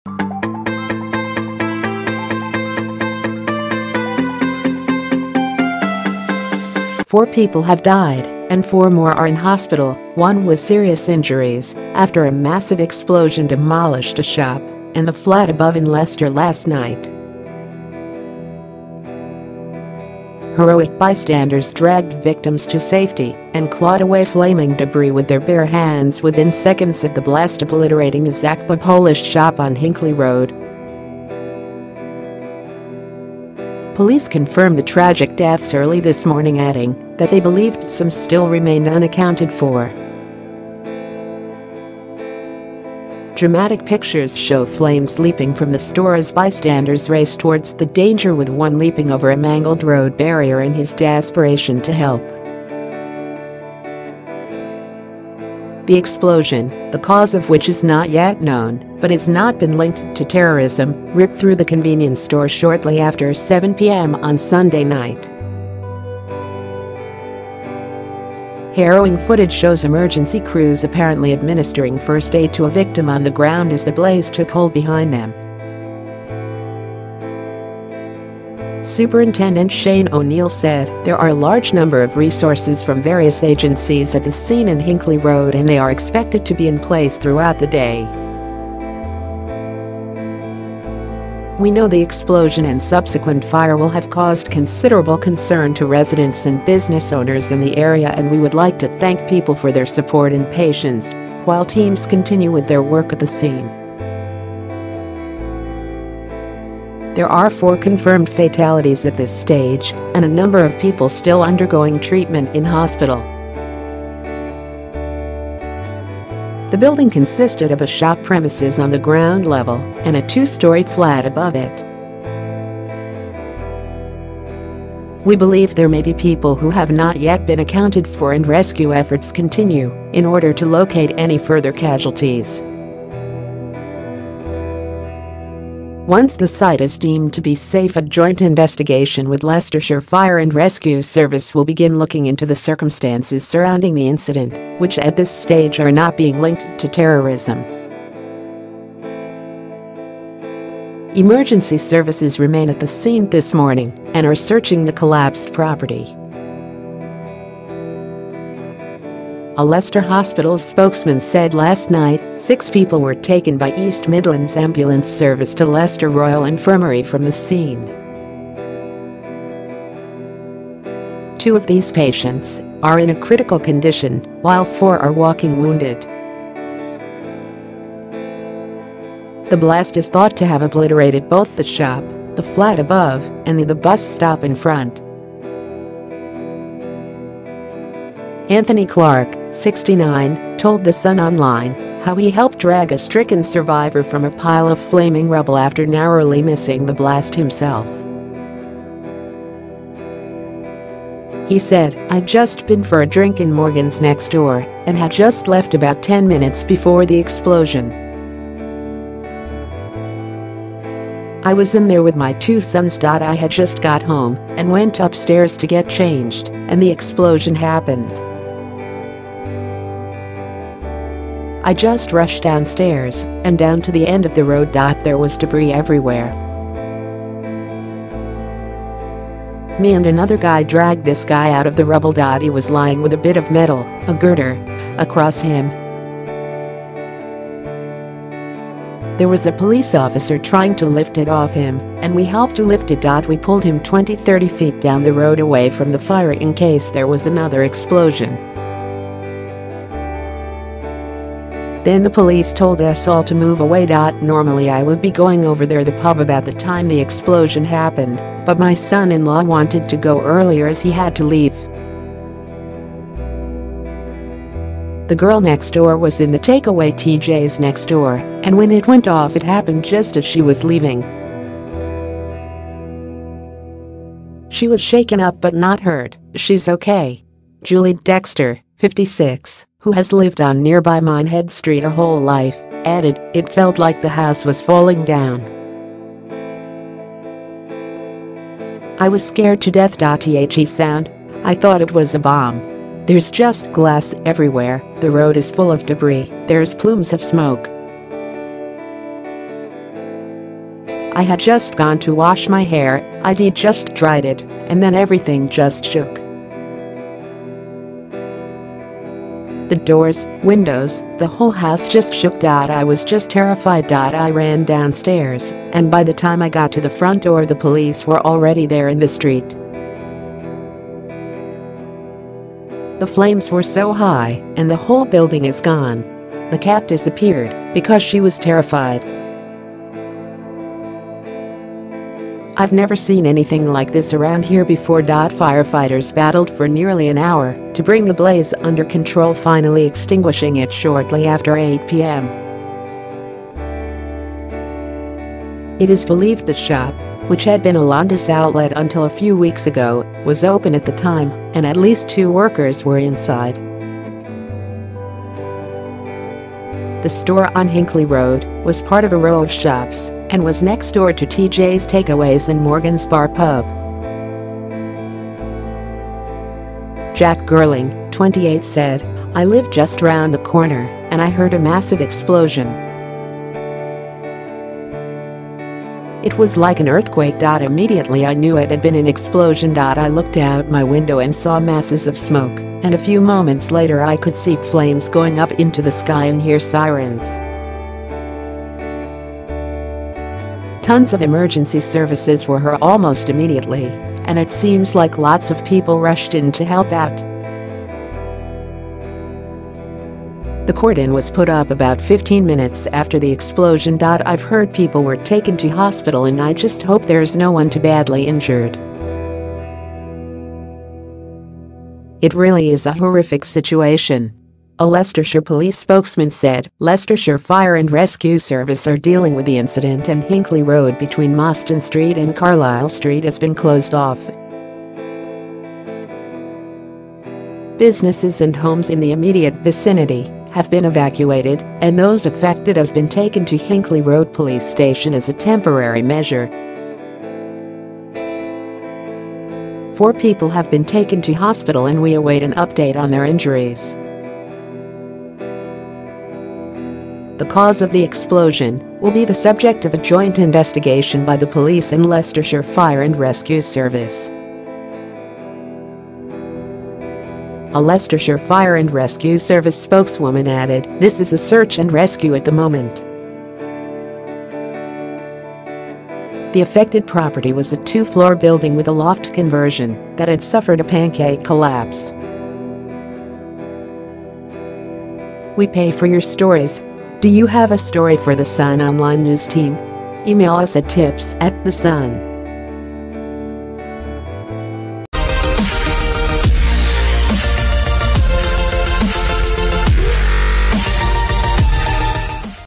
Four dead in massive explosion that destroyed shop and flat in Leicester - 247 News
FOUR people have died and four more are in hospital, one with serious injuries, after a massive explosion demolished a shop and the flat above in Leicester last night.Heroic bystanders dragged victims to safety and clawed away flaming debris with their bare hands within seconds of the blast obliterating the Zakba Polish shop on Hinckley Road.Police confirmed the tragic deaths early this morning adding that they believed some still remain unaccounted for.Dramatic pictures show flames leaping from the store as bystanders race towards the danger - with one leaping over a mangled road barrier in his desperation to help.The explosion, the cause of which is not yet known but has not been linked to terrorism, ripped through the convenience store shortly after 7pm on Sunday night.Harrowing footage shows emergency crews apparently administering first aid to a victim on the ground as the blaze took hold behind them.Superintendent Shane O’Neill said: “There are a large number of resources from various agencies at the scene in Hinckley Road and they are expected to be in place throughout the day.“We know the explosion and subsequent fire will have caused considerable concern to residents and business owners in the area and we would like to thank people for their support and patience while teams continue with their work at the scene.“There are four confirmed fatalities at this stage and a number of people still undergoing treatment in hospital."The building consisted of a shop premises on the ground level and a two-storey flat above it."We believe there may be people who have not yet been accounted for and rescue efforts continue in order to locate any further casualties.“Once the site is deemed to be safe a joint investigation with Leicestershire Fire and Rescue Service will begin looking into the circumstances surrounding the incident, which at this stage are not being linked to terrorism.” Emergency services remain at the scene this morning and are searching the collapsed property.A Leicester Hospitals spokesman said last night: "Six people were taken by East Midlands Ambulance Service to Leicester Royal Infirmary from the scene."Two of these patients are in a critical condition, while four are walking wounded." The blast is thought to have obliterated both the shop, the flat above, and the the bus stop in front.Anthony Clarke, 69, told The Sun Online how he helped drag a stricken survivor from a pile of flaming rubble - after narrowly missing the blast himself.He said: "I’d just been for a drink in Morgan’s next door and had just left about ten minutes before the explosion."I was in there with my two sons.I had just got home and went upstairs to get changed and the explosion happened."I just rushed downstairs and down to the end of the road.There was debris everywhere.Me and another guy dragged this guy out of the rubble."He was lying with a bit of metal, a girder, across him.There was a police officer trying to lift it off him and we helped to lift it."W